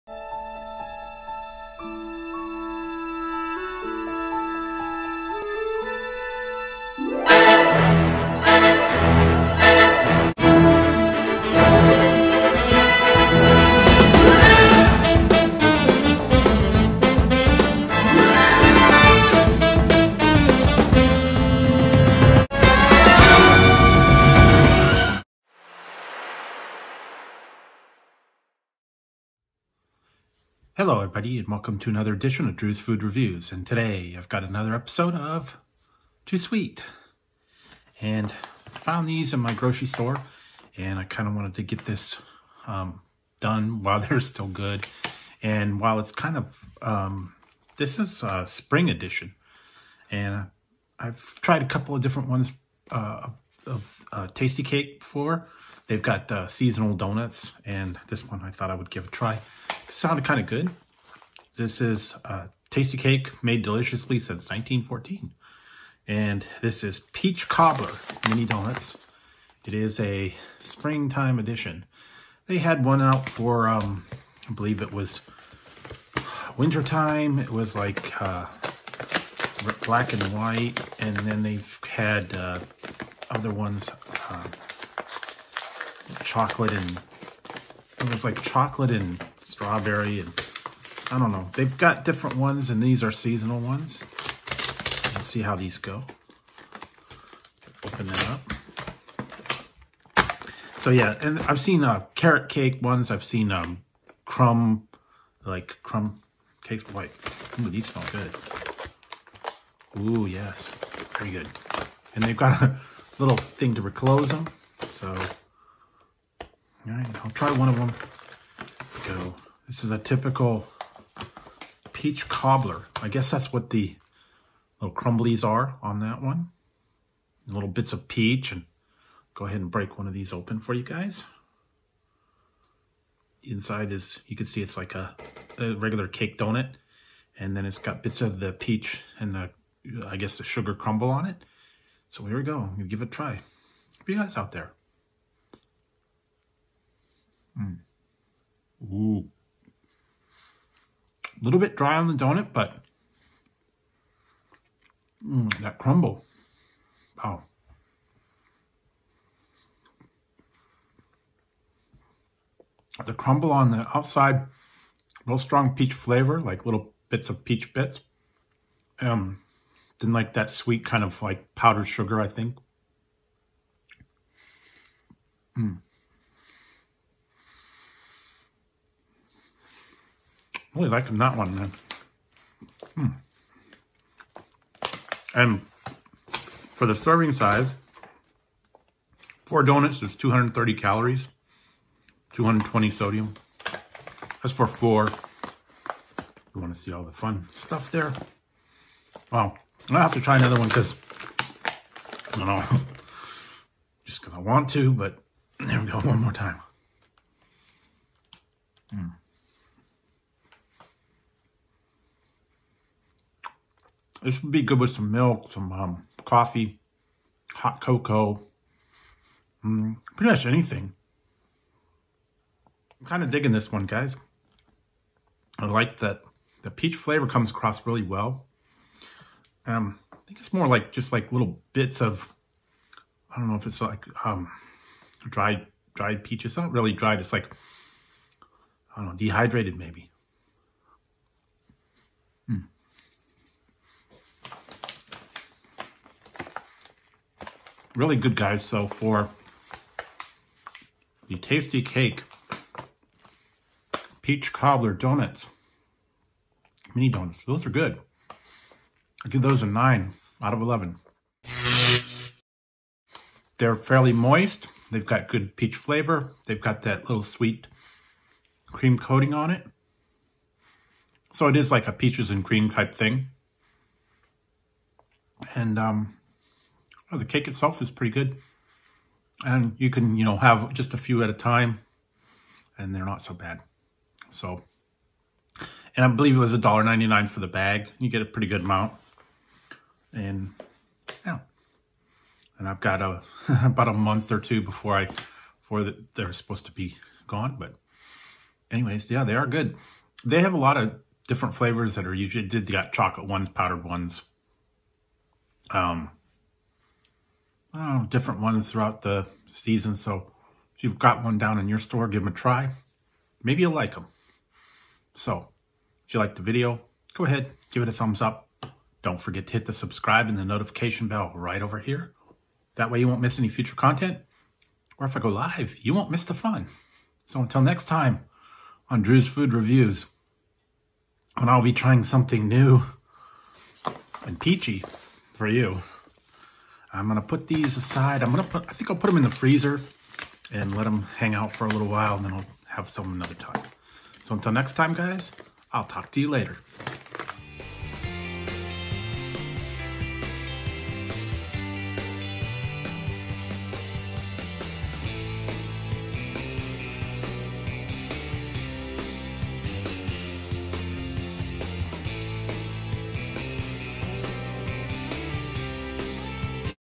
Too Sweet : Tastykake Peach Cobbler Donuts
Today on too sweet I’m trying Tastykake peach Cobbler Donuts let’s see how this one rates